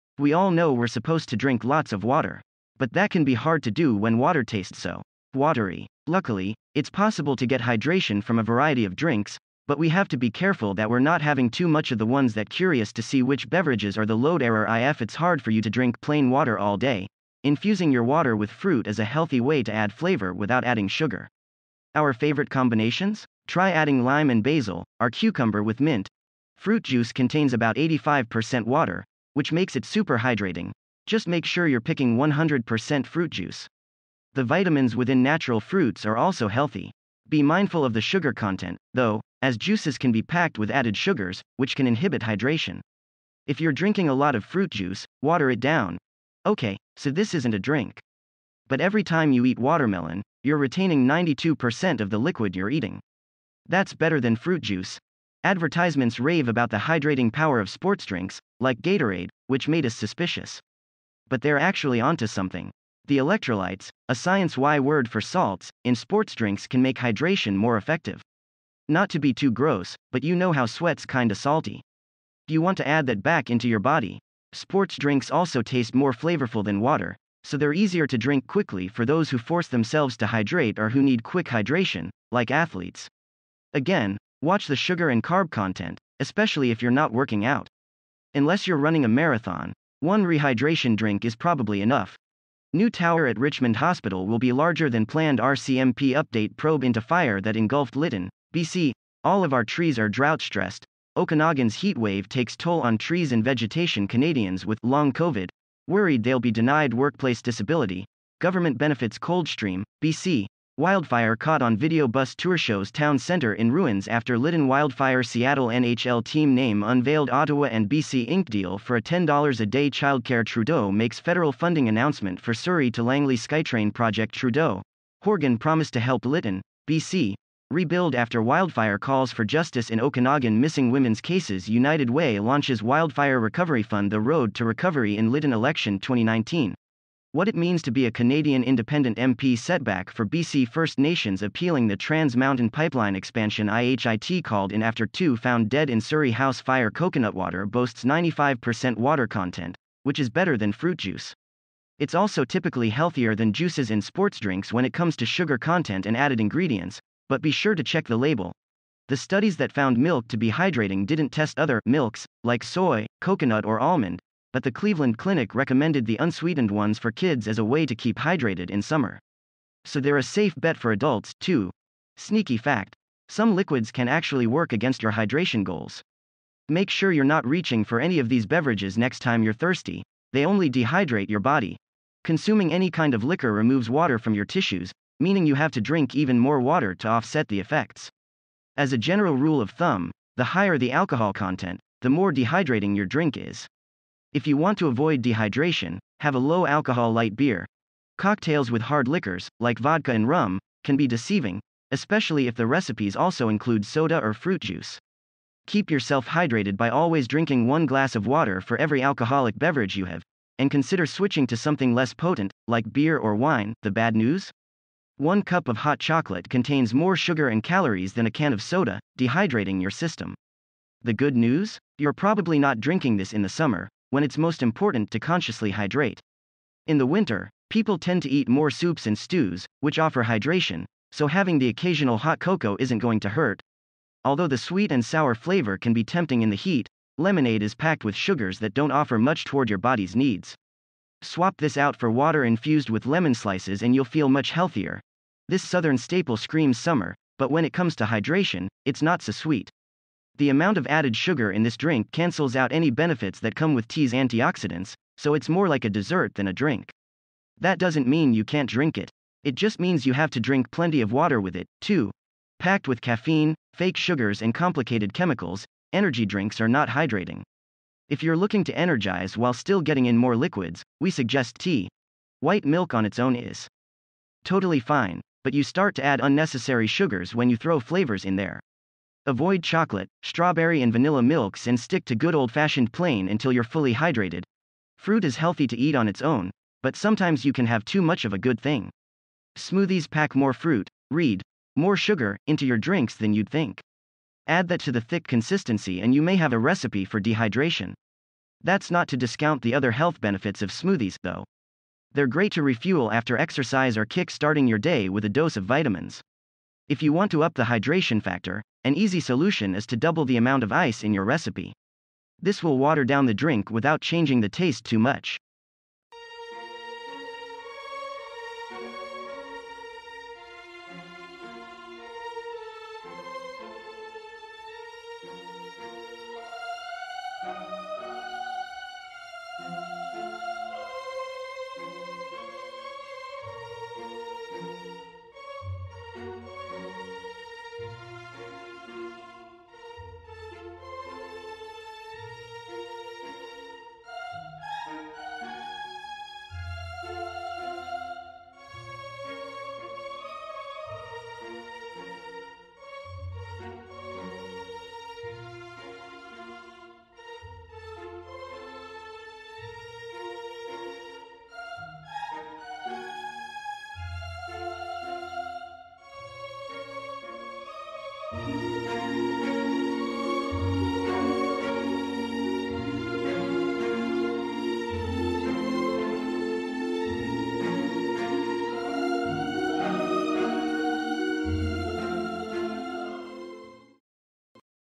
The best (and worst) drinks to keep you hydrated
We all know we're supposed to drink lots of water. But that can be hard to do when water tastes so... Watery. Luckily, it's possible to get hydration from a variety of drinks, but we have to be careful that we're not having too much of the ones thatCurious to see which beverages are theLoad [...]